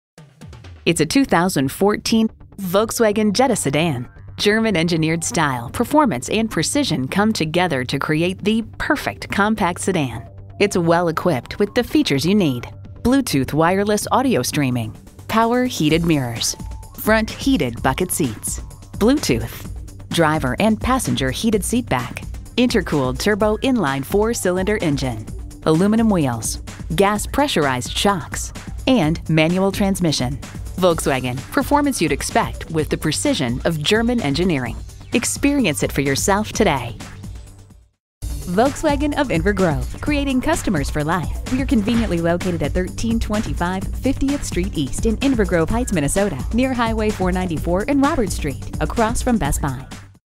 Used 2014 Volkswagen Jetta Sedan Saint Paul MN Minneapolis, MN G88670P - SOLD
Year: 2014
Make: Volkswagen
Model: Jetta Sedan
Trim: 4dr Man TDI 2.0L 4Cyl
Engine: 122 L 4 Cyl. Cyl.
Transmission: 6-Speed Manual
Color: Reflex Silver Metallic
Interior: Titan Black
Mileage: 32763
Stock #: G88670P
VIN: 3VW3L7AJXEM420239

Address:
1325 50th Street East